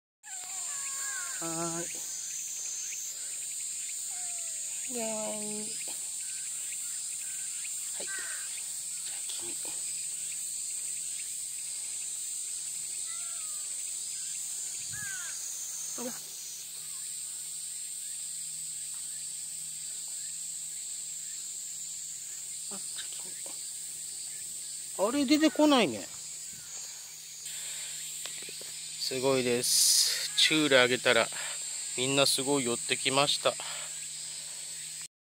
城ヶ島公園のねこさん達〜ねこパラダイス(=^・^=)
城ヶ島公園で暮らしてるねこさん達です(=^・^=)